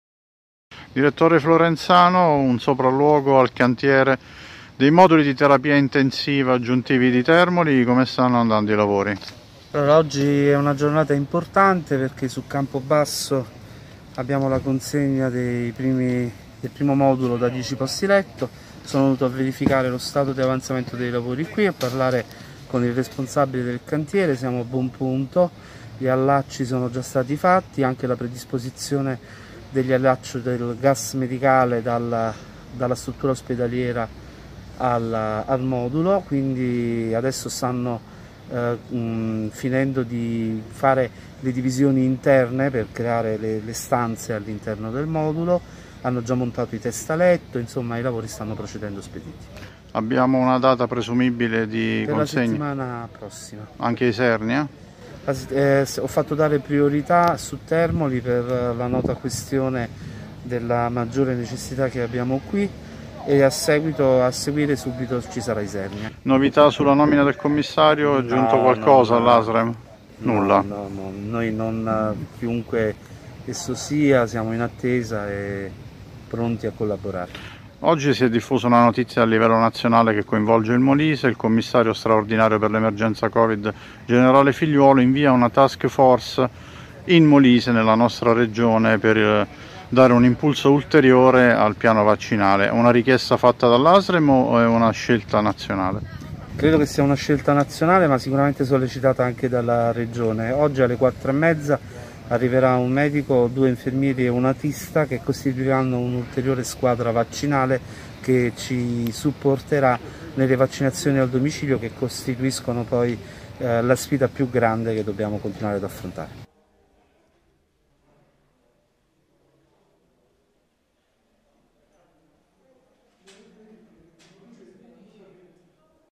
Terapia intensiva, blitz di Florenzano a Termoli. Vaccinazioni, Figliuolo invia ...
Servizi TG | Terapia intensiva, blitz di Florenzano a Termoli. Vaccinazioni, Figliuolo invia task force | 25/3/2021